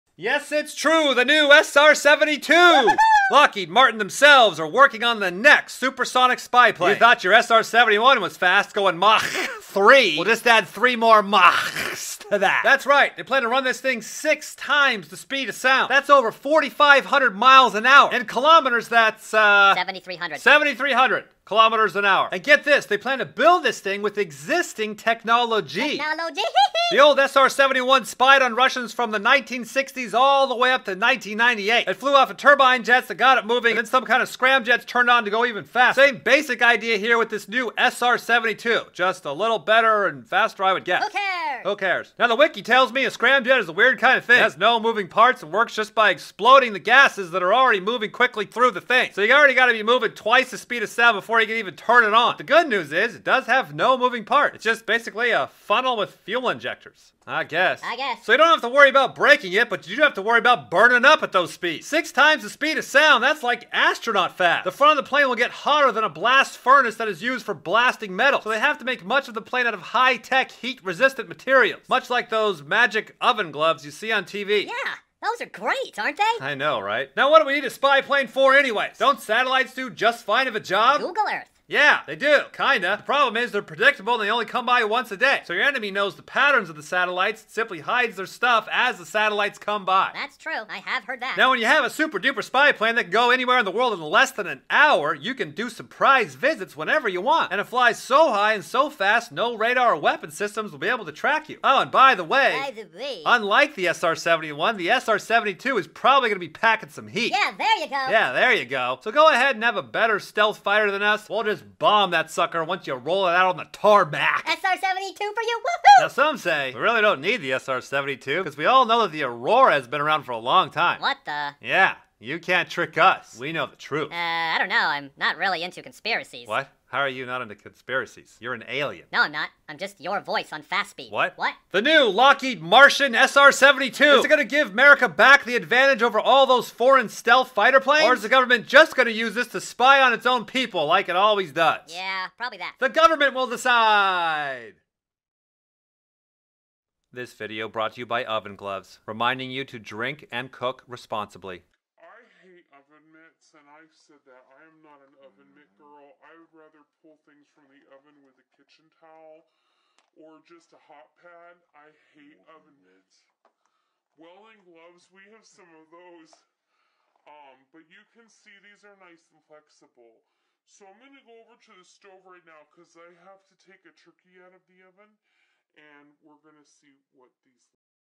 SR-72 MACH 6!
Lockheed's making the next supersonic spyplane that will go 6 times the speed of sound to spy on Russians in the morning and be back by dinner!